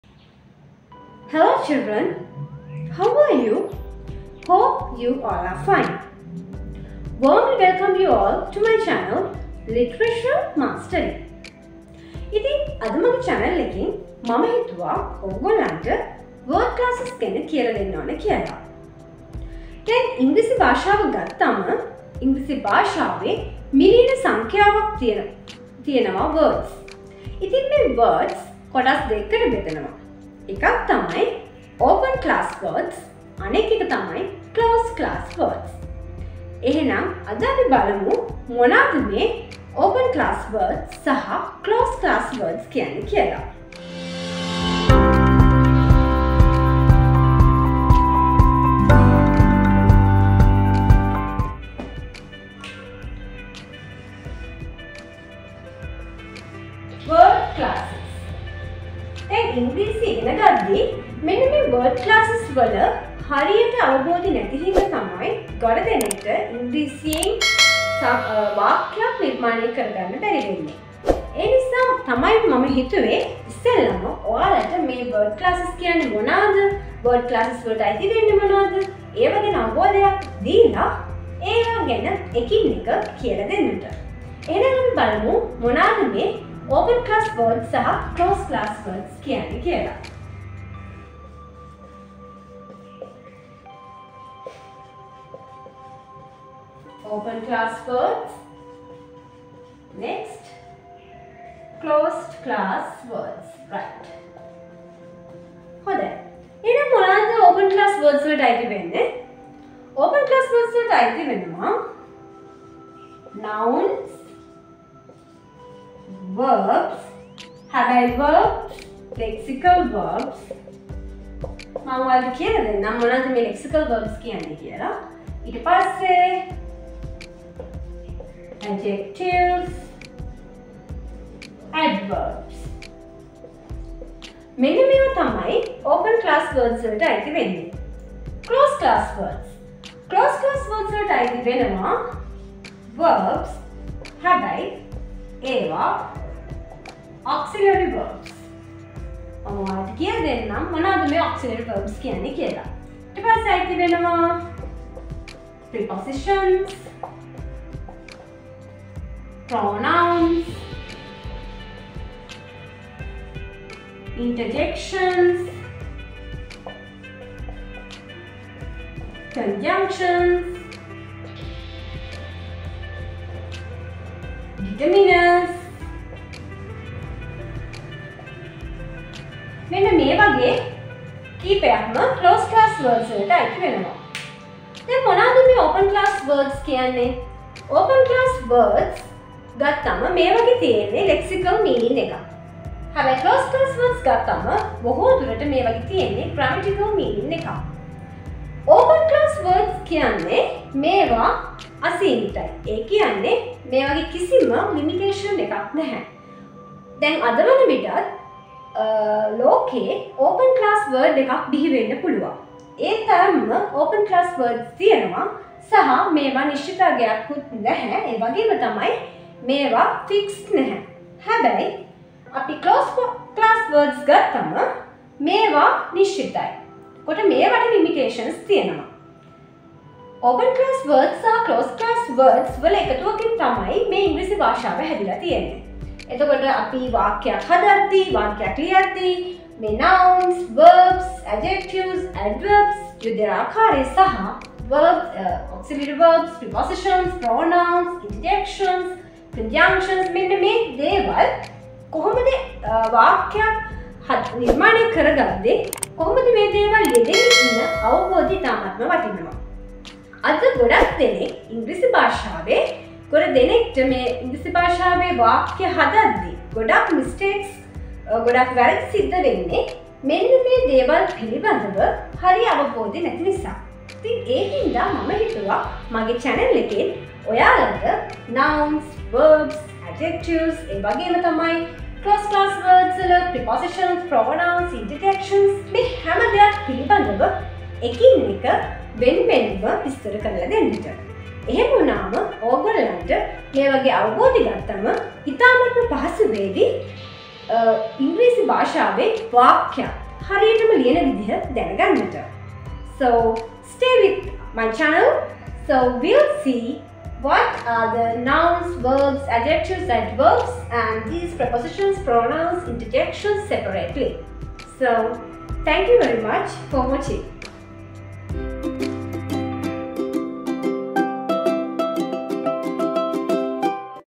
What are these Word Classes | මොනවද මේ Word Classes | Literature Mastery
Hello, I am Kusala Karunarathne and currently working as a national school English teacher.

Here I am going to begin lessons on English Language as well by supporting you to get through English successfully.